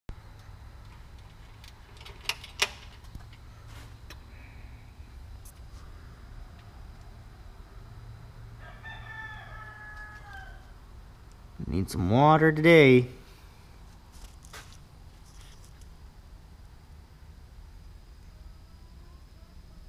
Day 5 progress pepper plant seedlings
Day 5 progress report on pepper plant seedlings. They will need to be watered today.